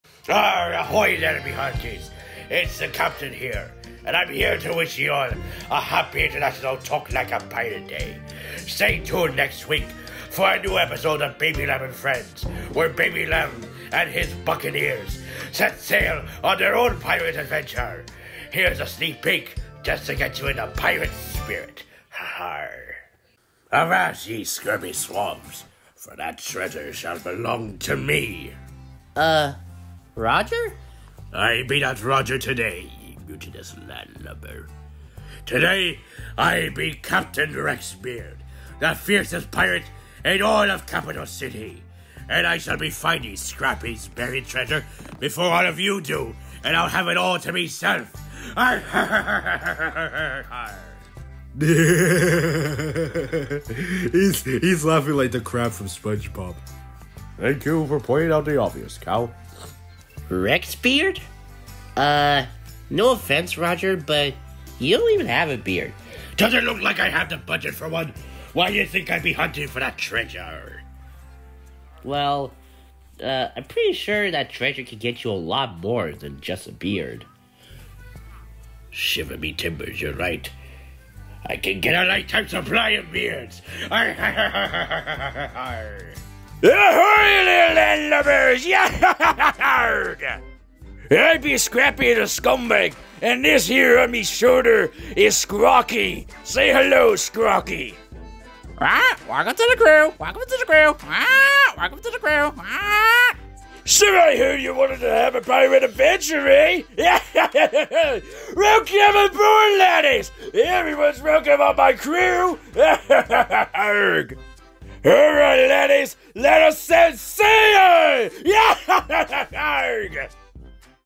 Talk Like a Pirate Day 2024!
Happy International Talk Like a Pirate Day, mateys!

In celebration of this funny parody holiday that's existed since 1995, I am showing ye a sneak peek of a swashbucklin' new episode of Baby Lamb & Friends that's a sequel to an early episode where Baby Lamb and his band of buccaneers set off on a pirate adventure!

Featuring my good friend @CanadianNic as the voices of Scrappy the Scumbag and Squawky the Parrot.